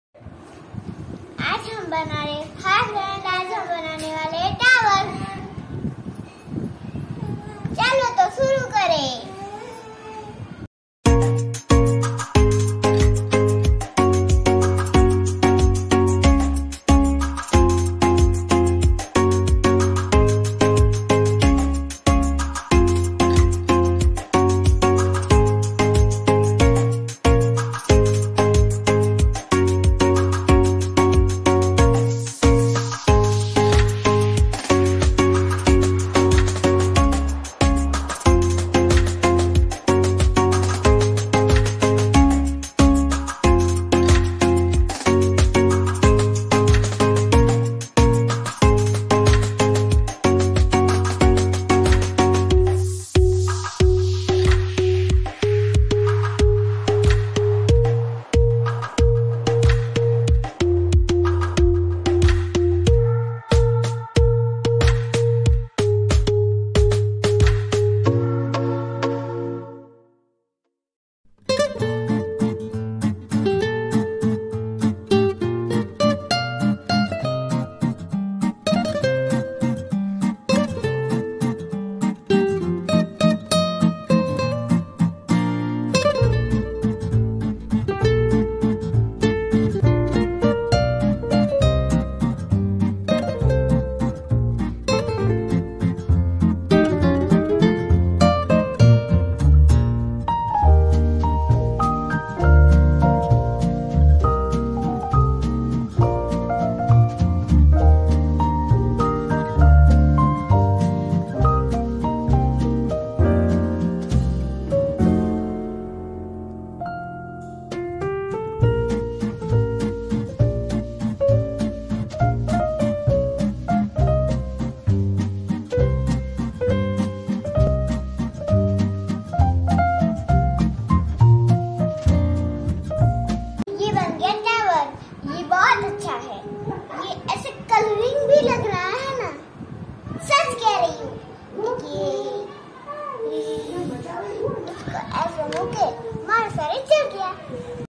how to make Eiffel Tower in building blocks at home | Block Games
New creativity with building blocks at home by anokhi creativity.